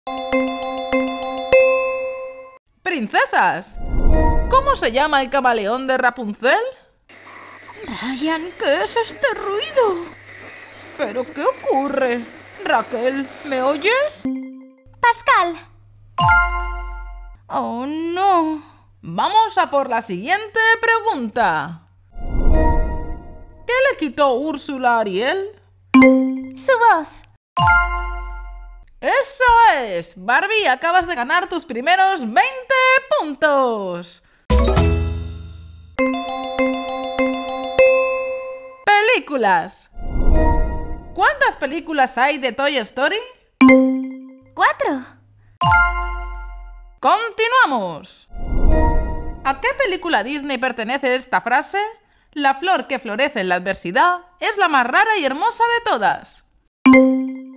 Barbie Participa en un Concurso de Cultura General 📚 Barbie TV Show parte 5 Cat Juguetes
Barbie participa en un programa de TV 📺 Barbie TV SHOW short parte 5 😸 Cat Juguetes